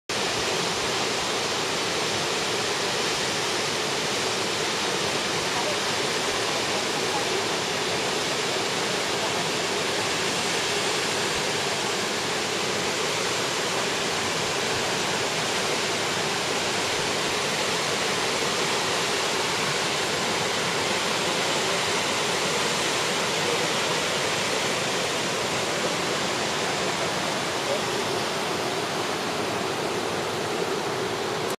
La catarata mas alta del mundo, "El salto del angel".
El salto del angel es una de la catarata mas alta del planeta, con una distancia de casi 1kilometro de altura tiene el placer de sostentar el record mundial de la catarata mas alta del mundo, pero no mui lejos le sigue su hermana gemela, lee el articulo que sigue a este video